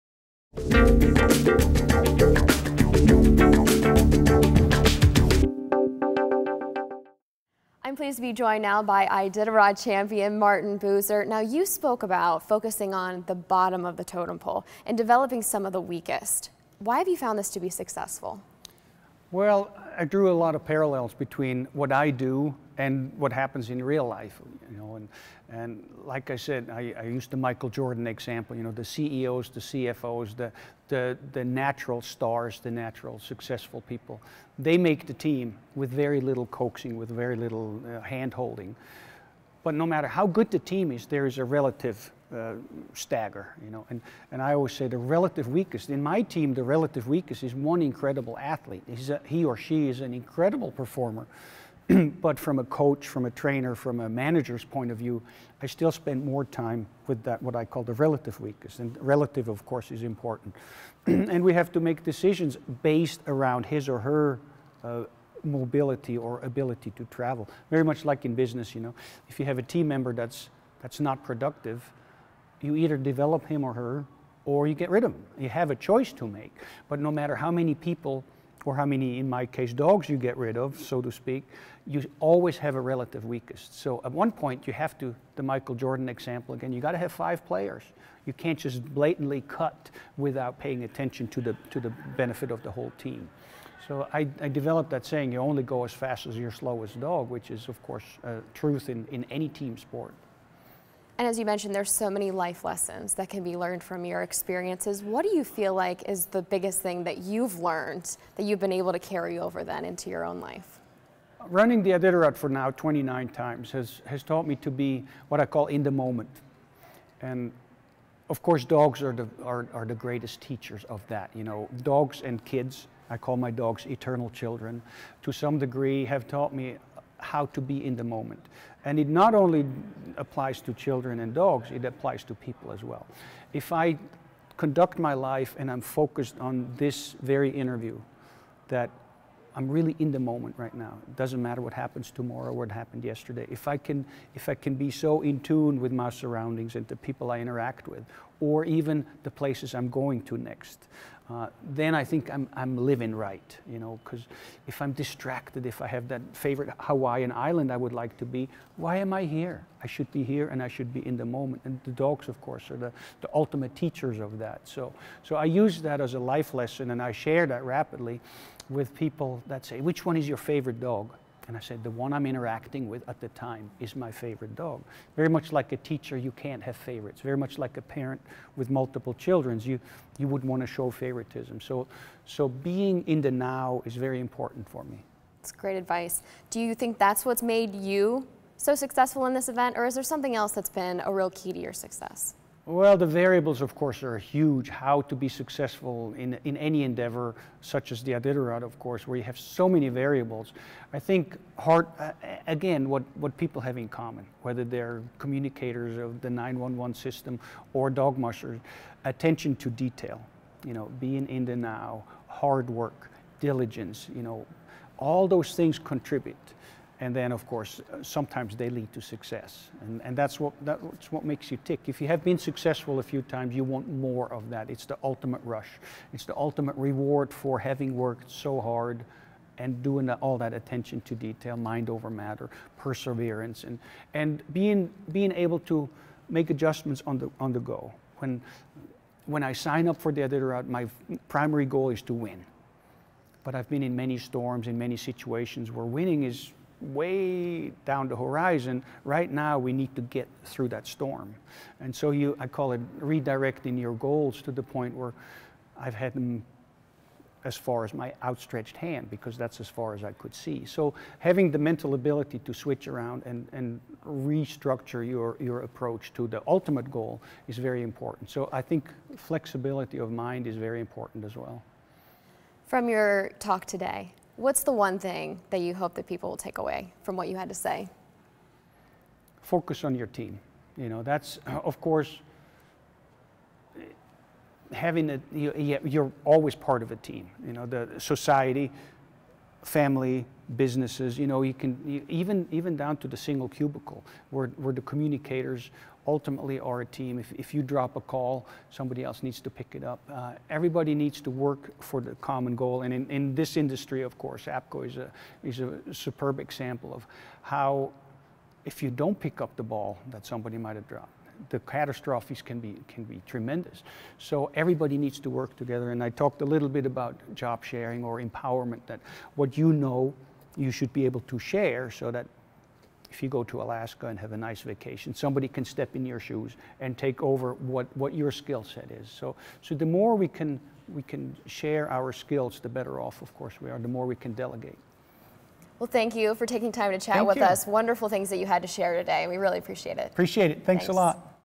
Interview with Martin Buser, Iditarod Champion
Four-time Iditarod Champion Martin Buser spoke with APCO TV's Amy Rogers about the life lessons he's acquired thru his competitive experiences that can be applied in all walks of life.